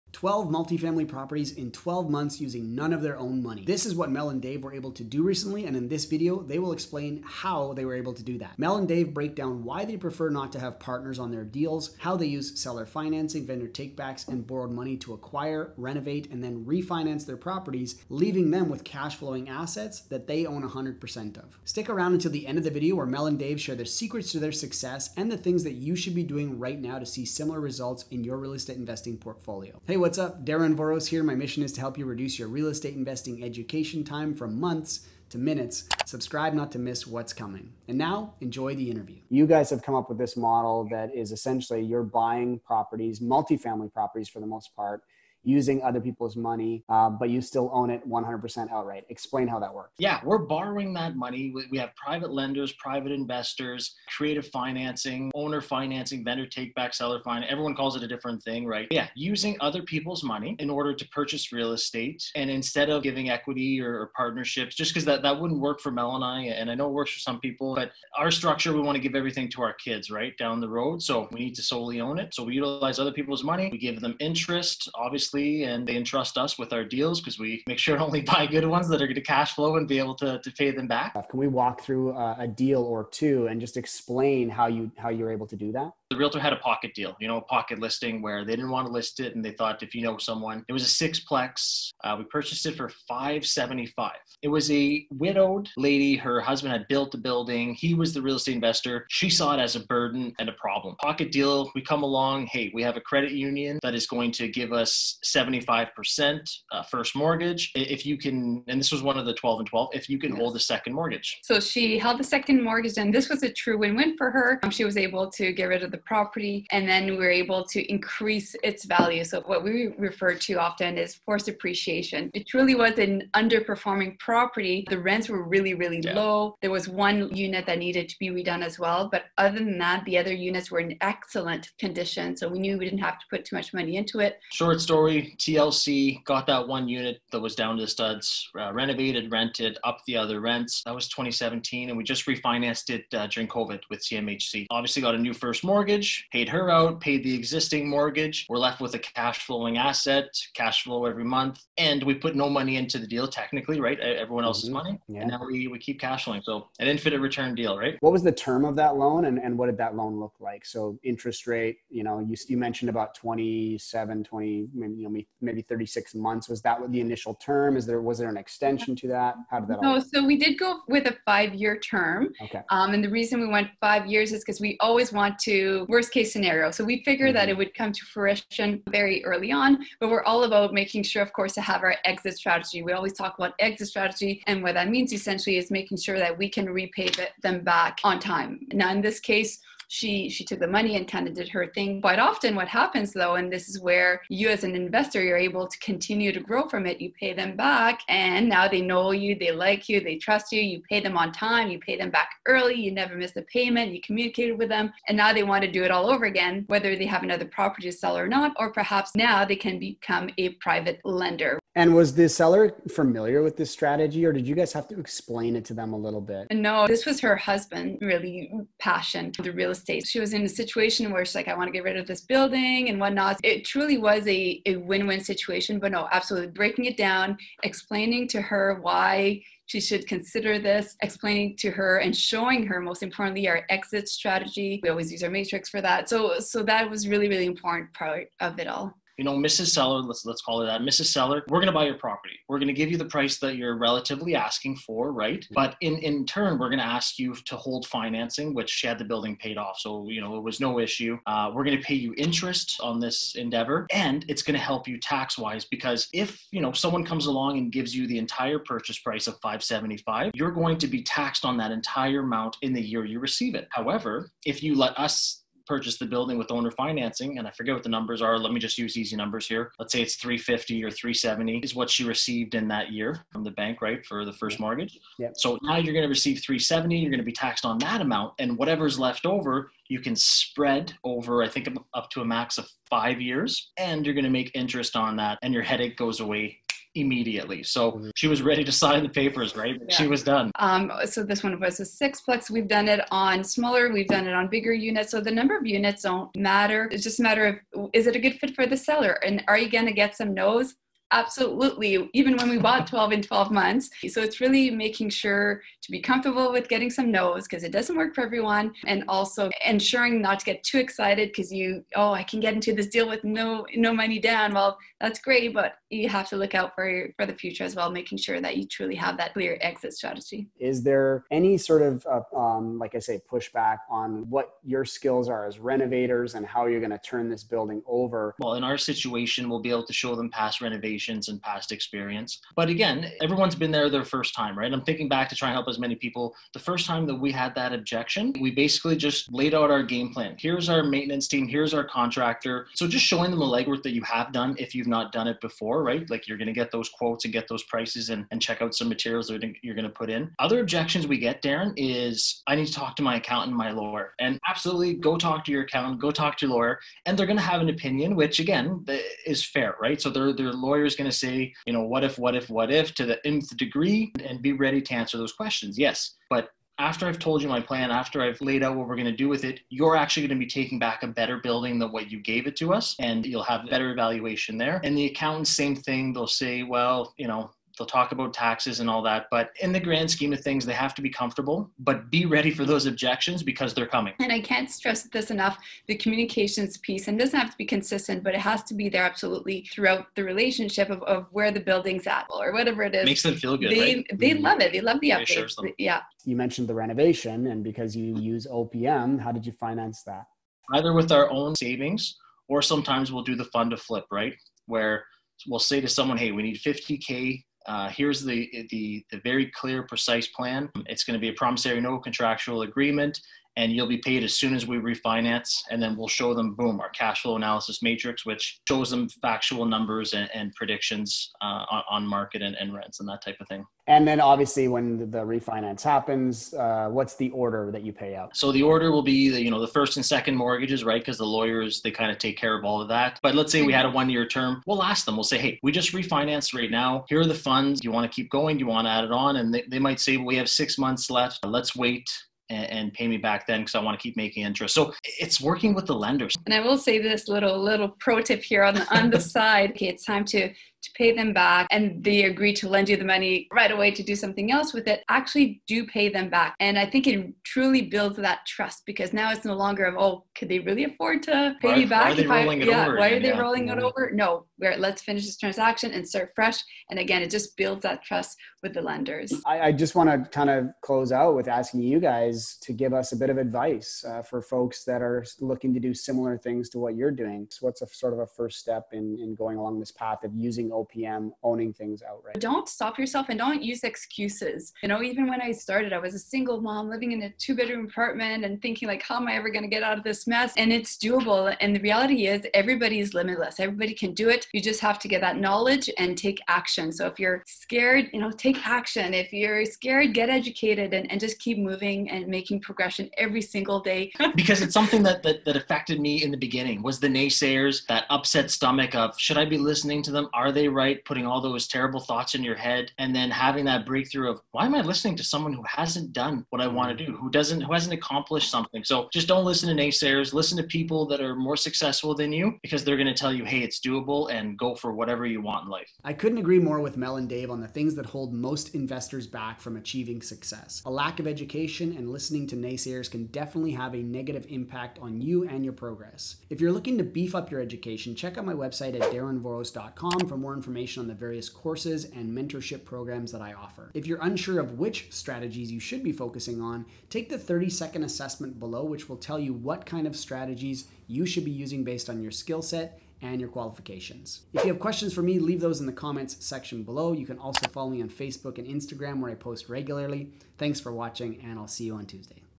Buying Apartment Buildings with No Money Down Canada
✅ TAKE THE ASSESSMENT: What kind of real estate investor are you?

Time Map:
00:00 Buying Apartment Buildings with No Money Down
00:30 My Mission
00:39 Buying Properties using other People’s Money
01:21 Pocket Deal
02:38 Five Year Term and Exit Strategies
03:59 Hold Financing
05:20 Push Backs as Renovators
06:36 How to Finance a Renovation
07:05 Orders of Payout
07:50 Advice for Aspiring Renovators
08:47 Take the 30 sec Assessment
09:14 Let’s Connect

Darren Voros